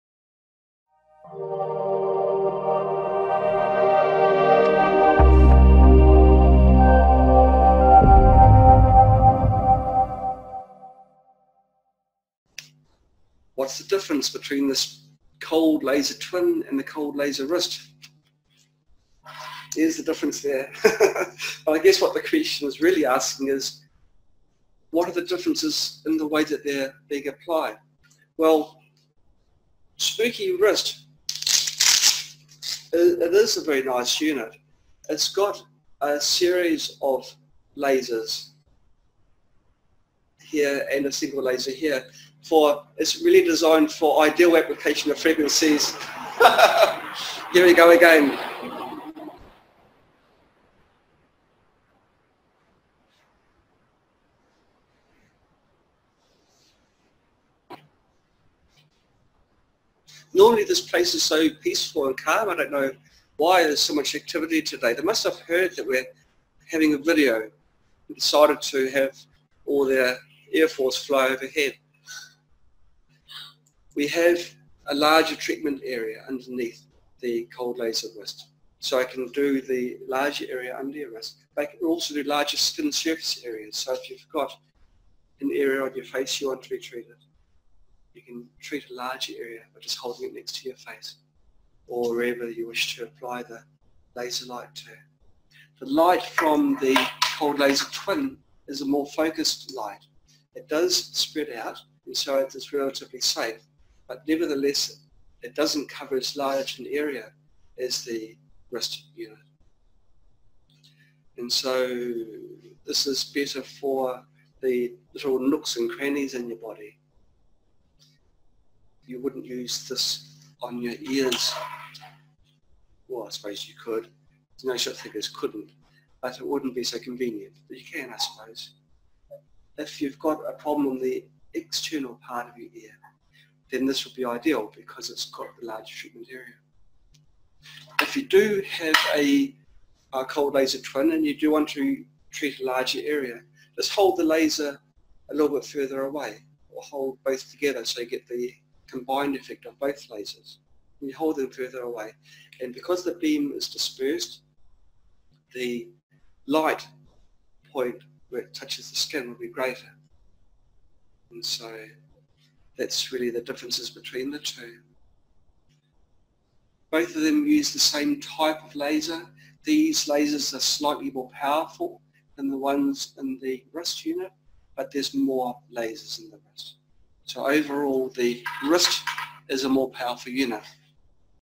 What’s the Difference Between the Cold Laser Twin and the Cold Laser Wrist?
- This video shows the differences between the Cold Laser Twin and the Cold Laser Wrist. The only differences are the number of lasers, power and physical construction. The wrist laser has the strap and body designed for easy treatments in wrist, leg or arm. And it is 5mw. Though the 5mW wrist unit has 5 lasers in total, so overall output power is higher (25mW). The twin has 2 more powerful lasers(7mw*2) that are better for applications in smaller areas such as ear, nose, navel or meridian treatments. Both can treat blood and use the same wavelength of light.